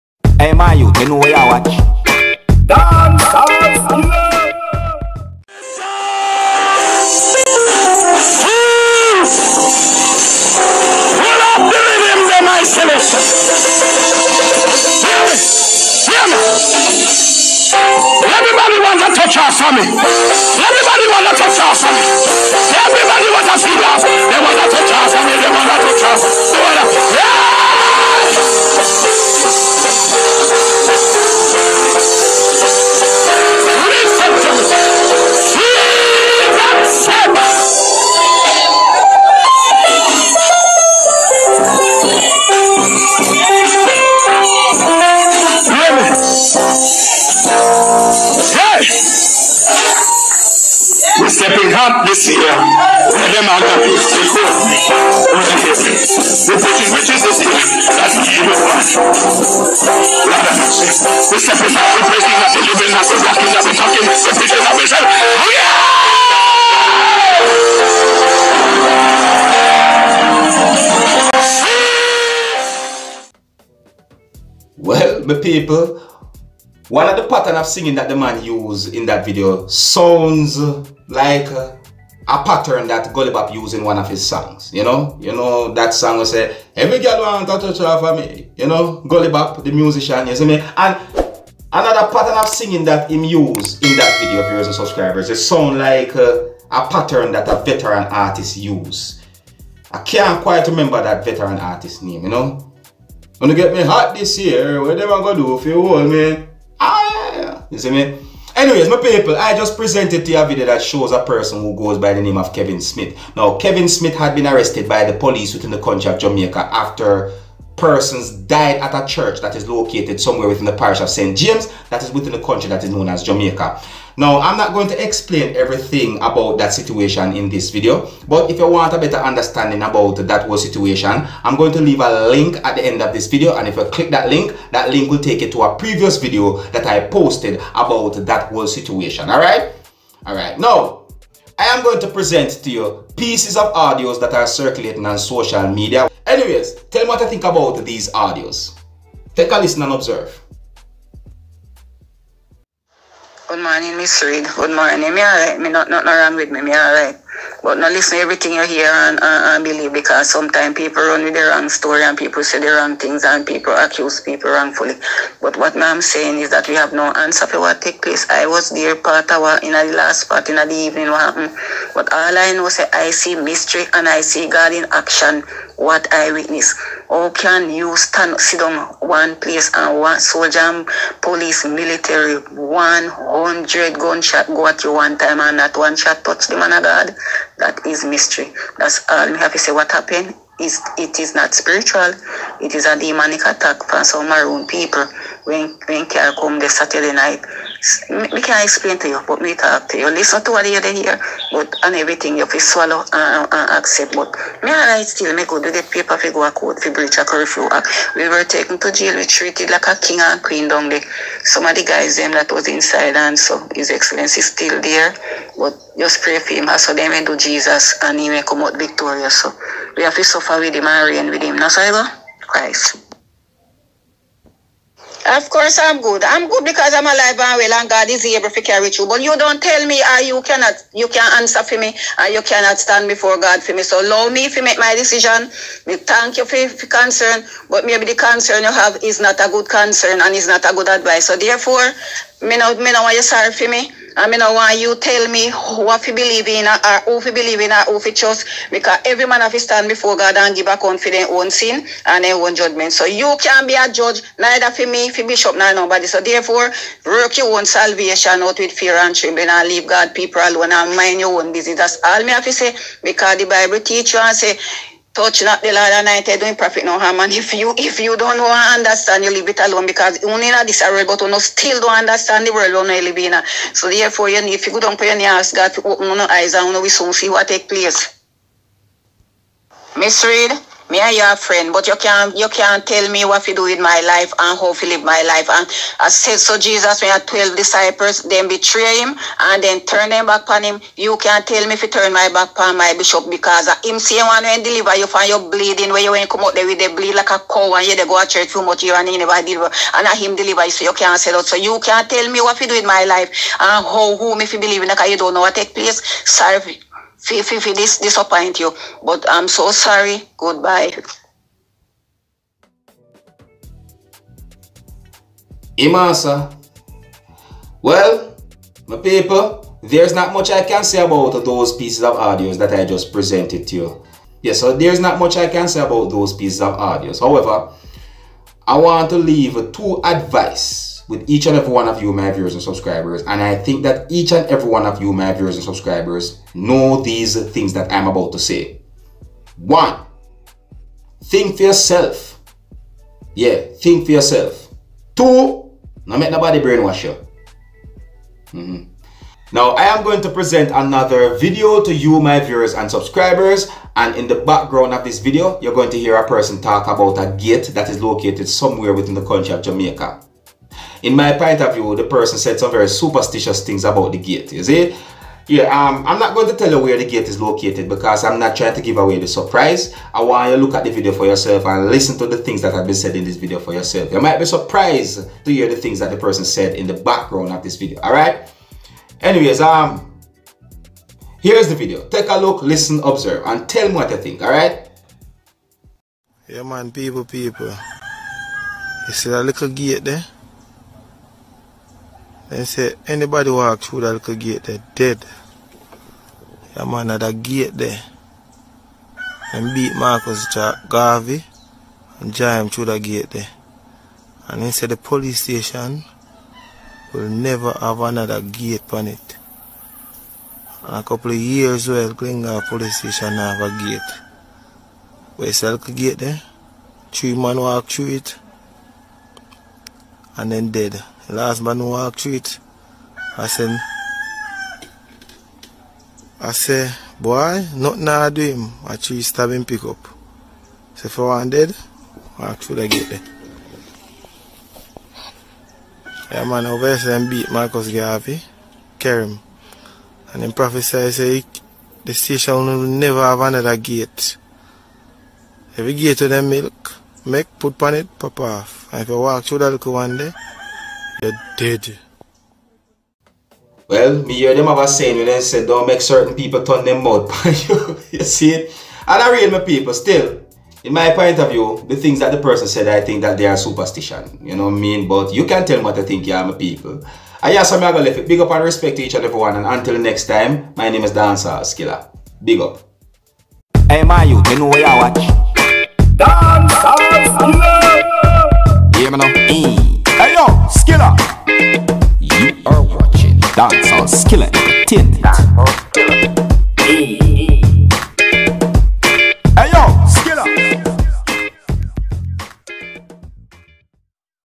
Church member Defend cult pastor, Audios allegedly from Kevin Smith church members
WARNING !
Rated - PG !

Welcome to dancehall skilla vlog.

be sure to know that all my videos are for you